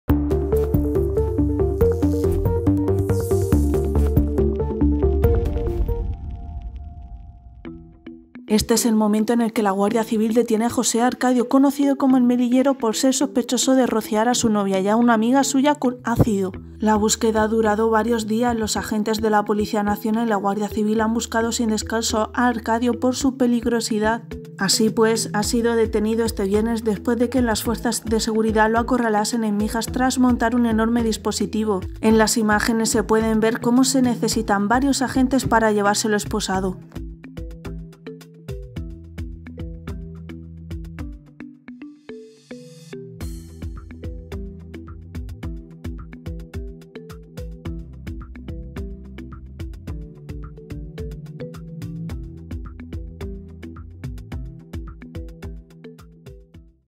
Momento de la detención de ‘El Melillero’
Este es el momento en el que la Guardia Civil detiene a José Arcadio, conocido como El Melillero, por ser sospechoso de rociar a su novia y a una amiga suya con ácido.

Locución y redacción: Alicia Martínez
Imágenes cedidas por Guardia Civil
Edición: Elena Campoy